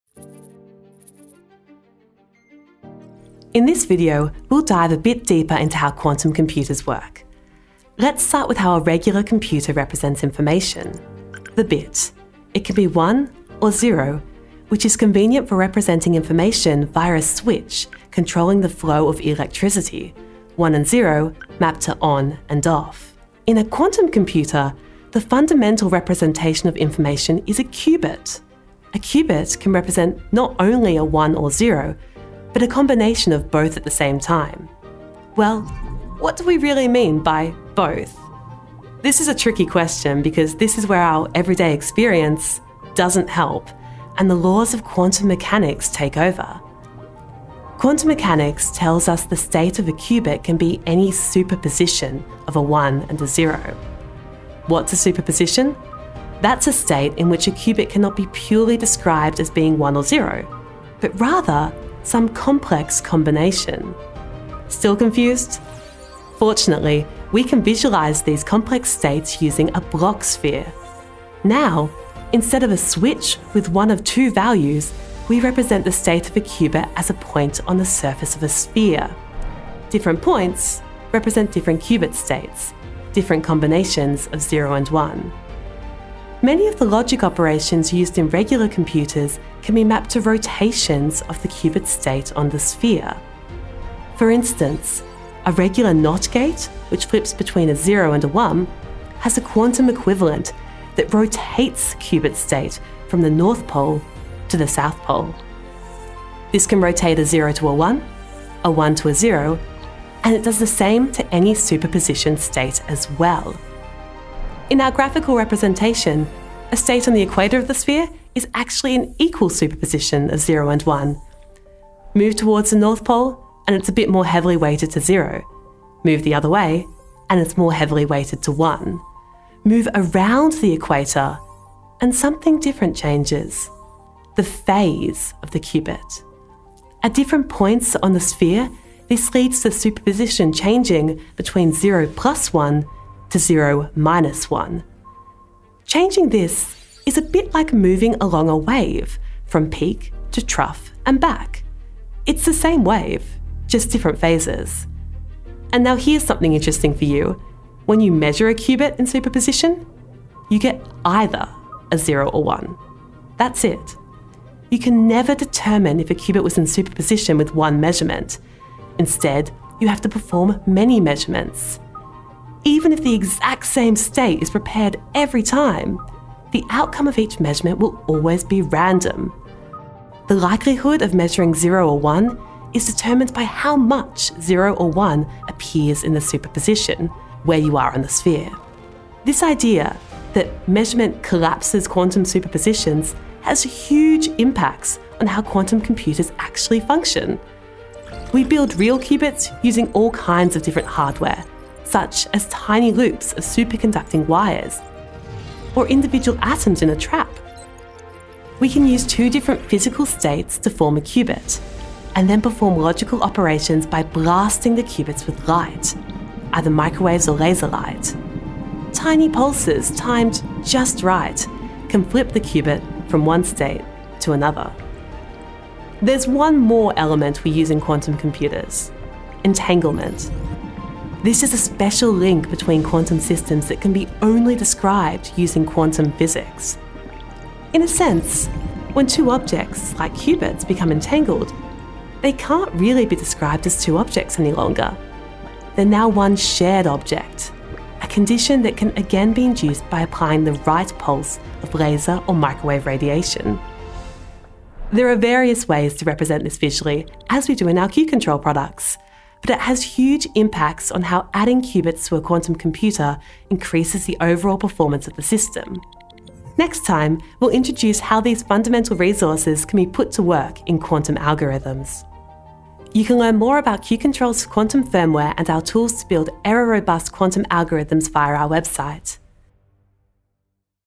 Quantum fundamentals (ep2)
In this video we dive a bit deeper into how quantum computers work. We discuss qubits and how we build them, superposition and entanglement.